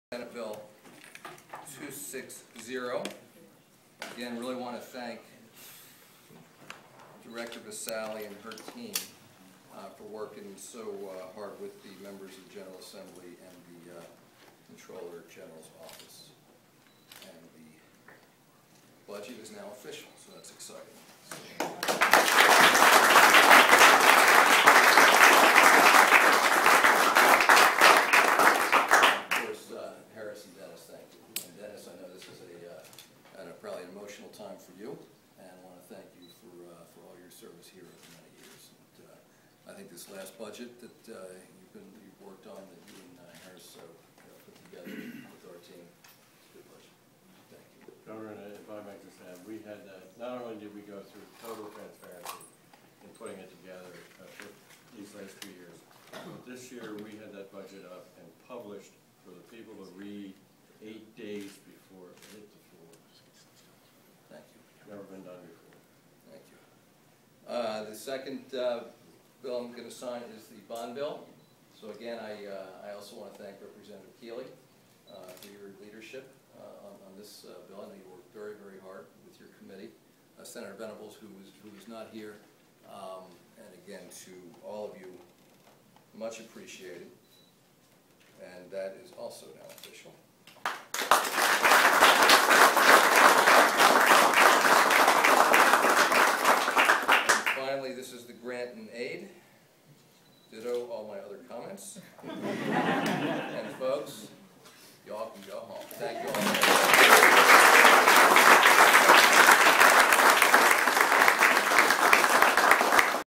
Budget Signing Delaware
At 1:30 a.m. July 1st in his Legislative Hall office, Governor Markell signed Delaware's FY 2013 Budget, Bond Bill and Grant-in-Aid.